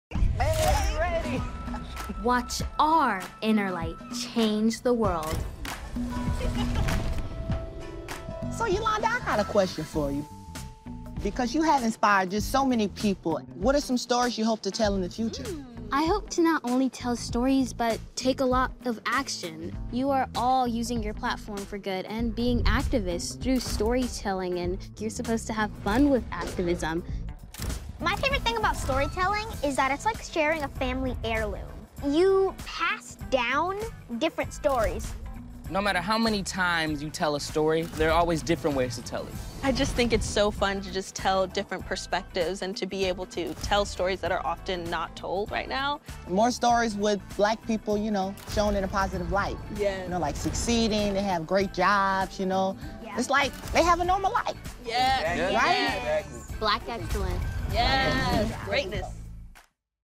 Celebrate Black History with Disney Channel Stars and Yolanda Renee King | Part 1
Watch our inner-light change the world! Yolanda Renee King collaborates with Christian J. Simon, Dara Reneé, Issac Ryan Brown and other Disney Channel stars to activate through storytelling and share your perspective!